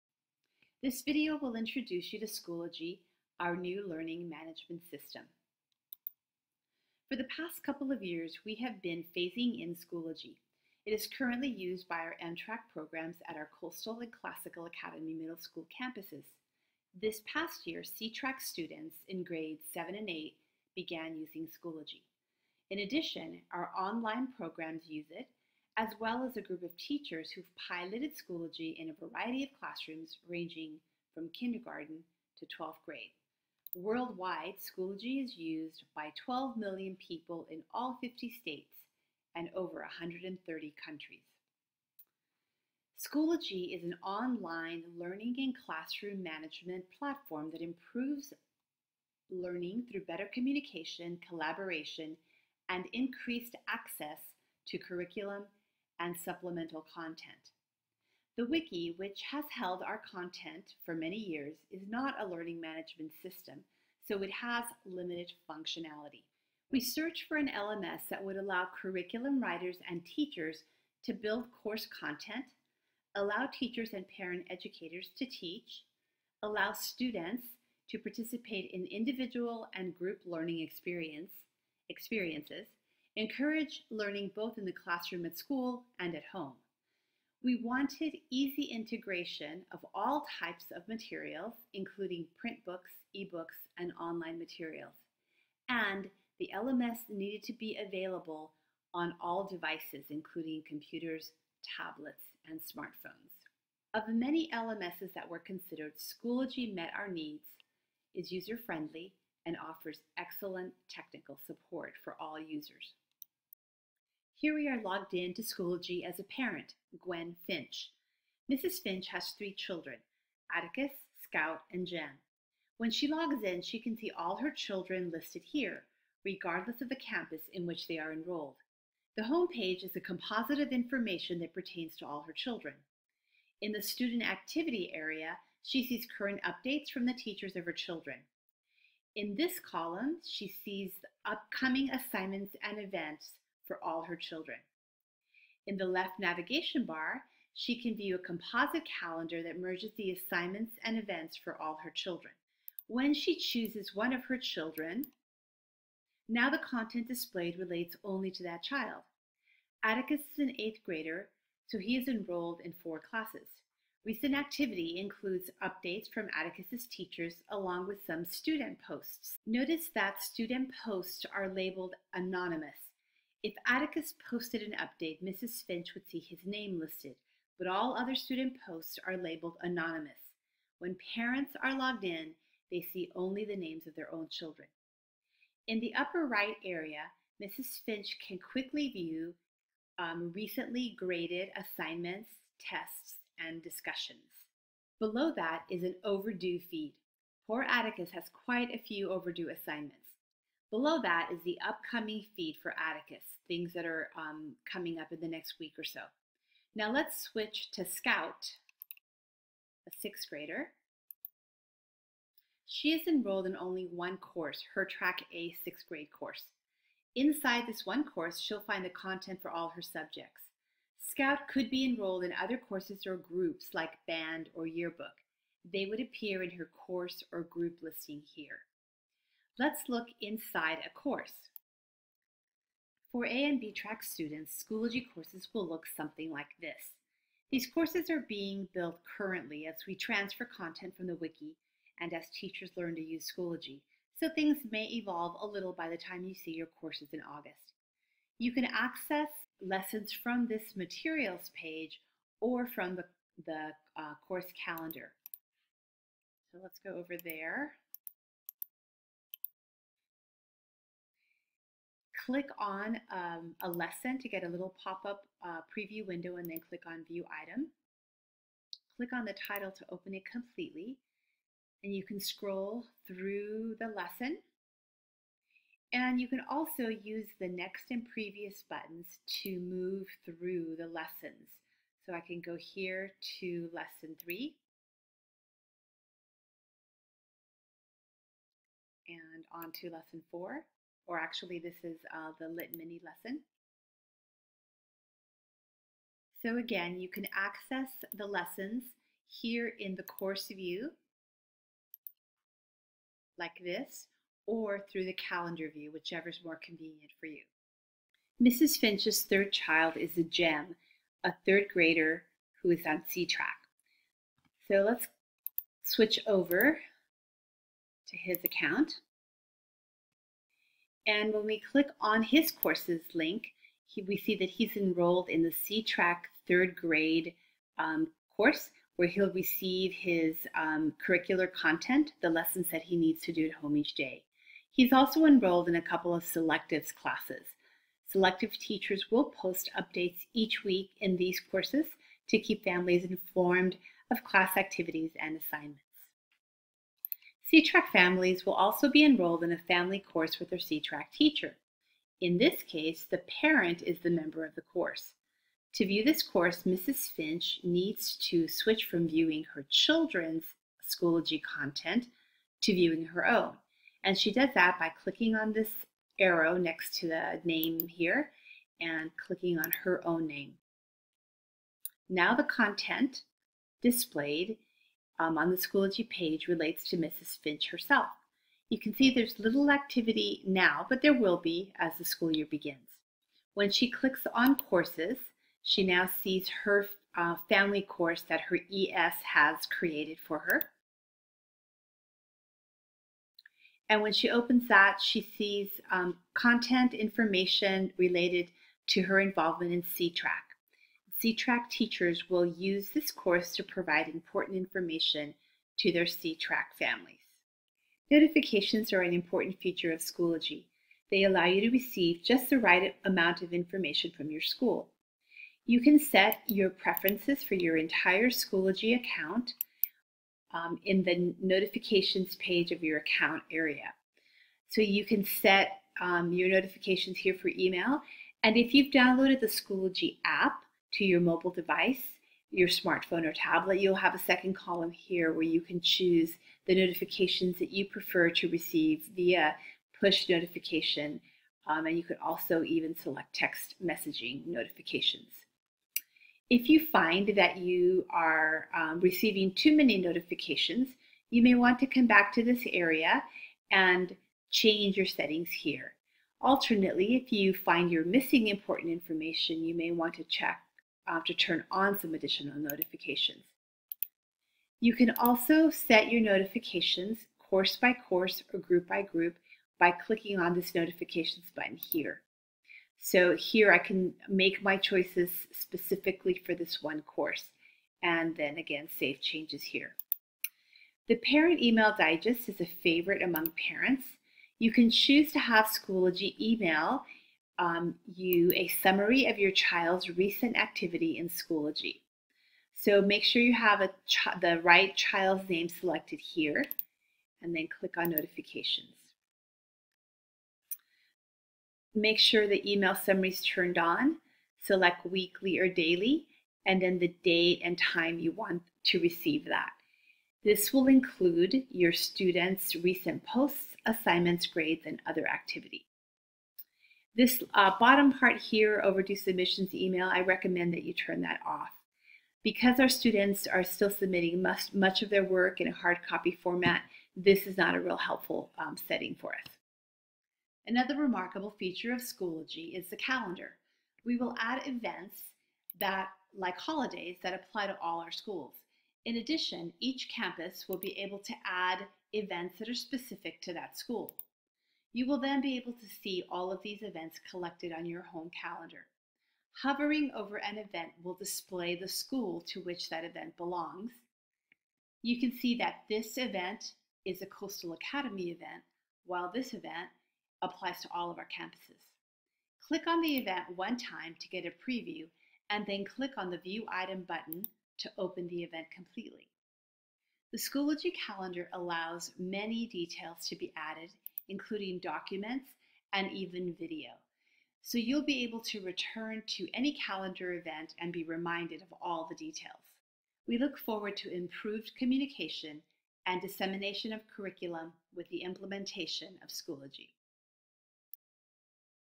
Schoology Demonstration for Parents
This is a Schoology video demonstration created for parents. Here are some highlights:
Intro
0:42 - Learning Management System defined
1:54 - How to view Schoology accounts for all your students at all campuses using your parent login.
2:35 - An 8th grader's view
3:50 - Inside a 6th grade course
5:08 - C Track 3rd grade student
6:47 - Setting notification preferences
8:01 - Parent Email Digest
8:50 - Schoology Calendar